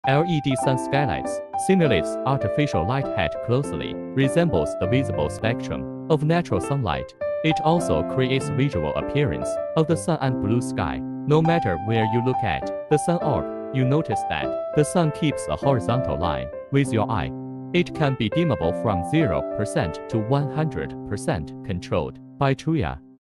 500W Artificial Skylight Blue Sky Sunlight Panel DIY 120X60 Vertical Multiscene
Artificial skylight is an optical system based on nano technology to artificially reproduce the natural light and visual appearance of the sun and sky. No matter where you look at the sun orb,you can notice that the sun keeps a horizontal line with your eye.

Rongyang OPT is a manufacturer of smart skylight in China.